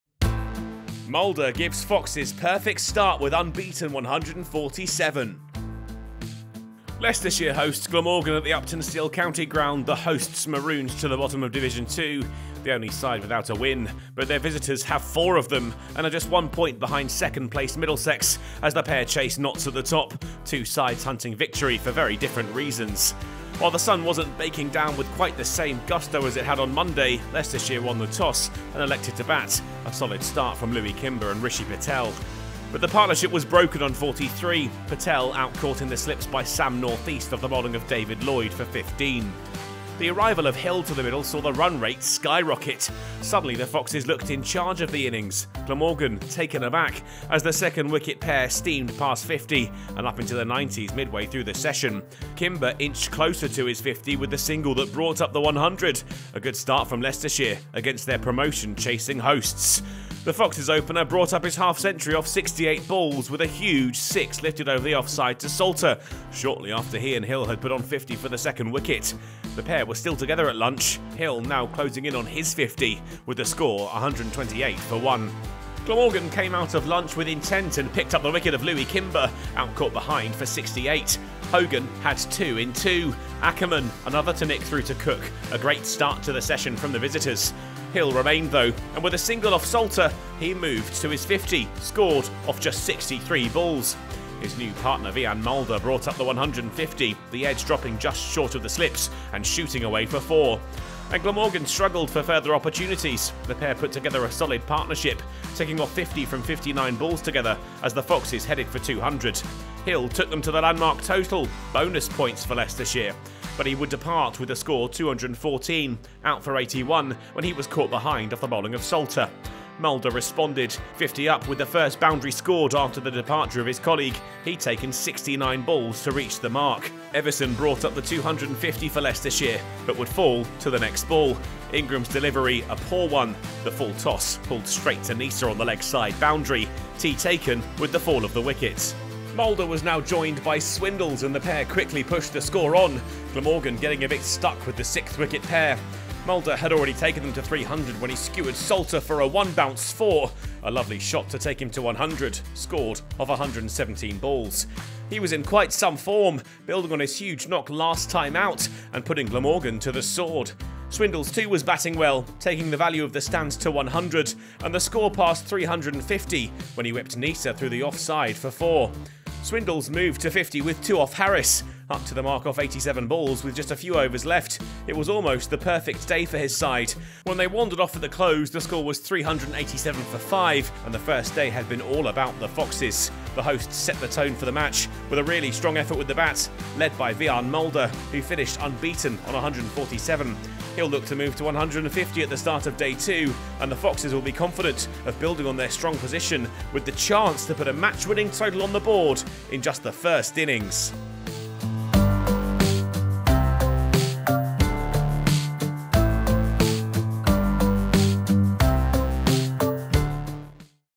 Mulder Ton Cements Foxes' Dominance - Day One Highlights
Wiaan Mulder's unbeaten century, alongside fifties from Harry Swindells, Lewis Hill and Louis Kimber helped establish a dominant first day for Leicestershire in their LV = Insurance County Championship fixture against Glamorgan at Uptonsteel County Ground.